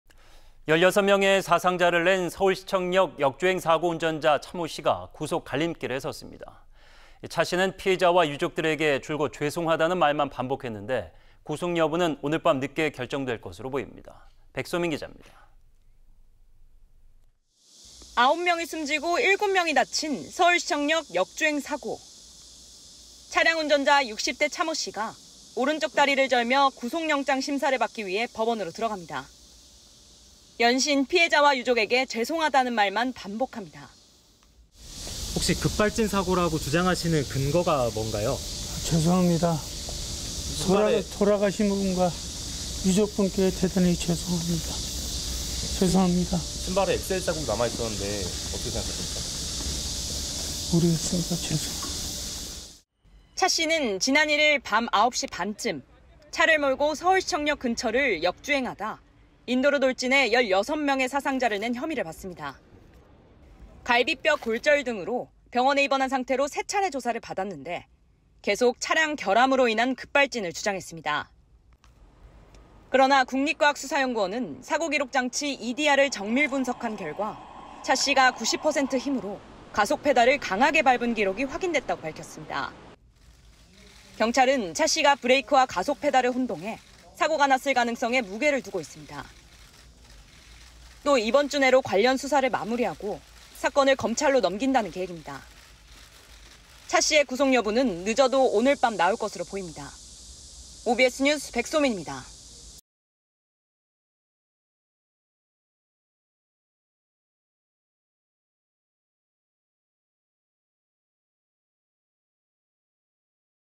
'시청역 역주행' 운전자 구속 기로..."돌아가신 분·유족께 죄송"
【앵커】
16명의 사상자를 낸 서울시청역
역주행 사고 운전자 차 모 씨가
구속 갈림길에 섰습니다.
차 씨는 피해자와 유족들에게
줄곧 죄송하다는 말만 반복했는데,
구속 여부는 오늘 밤늦게 결정될
것으로 보입니다.
 백소민 기자입니다.

【기자】

9명이 숨지고 7명이 다친 서울시청역 역주행 사고.

차량 운전자 60대 차 모 씨가 오른쪽 다리를 절며, 구속영장심사를 받기 위해 법원으로 들어갑니다.

연신 피해자와 유족에게 죄송하다는 말만 반복합니다.

[차 모 씨 / 서울시청역 역주행 사고 운전자 : (혹시 급발진 사고라고 주장하시는 근거가 뭔가요?) 죄송합니다. 돌아가신 분과 유족분께 대단히 죄송합니다. 죄송합니다. (신발에 액셀 자국이 남아 있었는데 어떻게 생각하십니까?) 모르겠습니다. 죄송합니다.]

차 씨는 지난 1일 밤 9시 반쯤 차를 몰고 서울시청역 근처를 역주행하다 인도로 돌진해 16명의 사상자를 낸 혐의를 받습니다.

갈비뼈 골절 등으로 병원에 입원한 상태로 3차례 조사를 받았는데, 계속 차량 결함으로 인한 급발진을 주장했습니다.

그러나 국립과학수사연구원은 사고 기록 장치 EDR을 정밀 분석한 결과,

차 씨가 90% 힘으로 가속 페달을 강하게 밟은 기록이 확인됐다고 밝혔습니다.

경찰은 차 씨가 브레이크와 가속페달을 혼동해 사고가 났을 가능성에 무게를 두고 있습니다.

또 이번 주 내로 관련 수사를 마무리하고, 사건을 검찰로 넘긴다는 계획입니다.

차 씨의 구속여부는 늦어도 오늘 밤 나올 것으로 보입니다.

 OBS뉴스 백소민입니다.